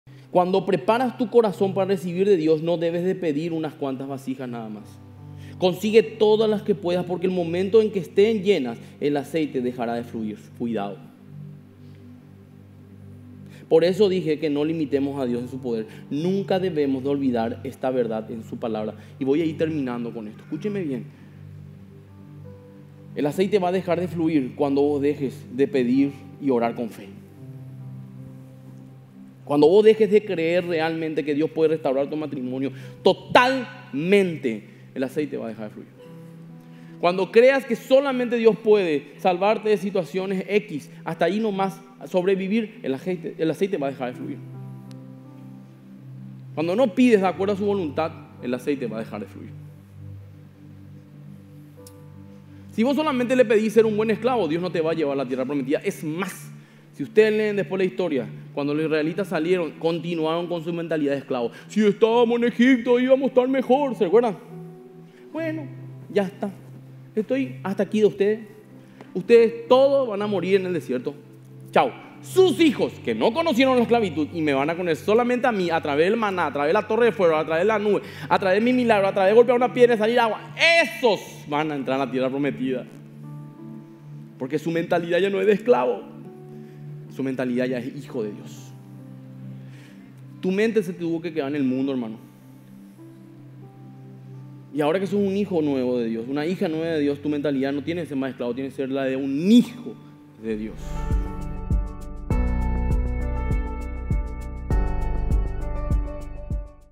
MC 017 Mensaje corto MQV - Mentalidad de esclavos
No intentemos limitar a Dios en su poder, el aceite dejará de fluir cuando dejes de pedir y orar con fe. Muchas veces una mentalidad de esclavo y no de hijo, nos impedirá ver el verdadero poder de nuestro Dios.

Fragmento de la prédica “Orando y pidiendo con fe” del pastor Adolfo Aguero Esgaib a ser emitida este domingo 20 de enero a las 10:00 de la mañana por la RPC (Canal 13). ¡Compártelo!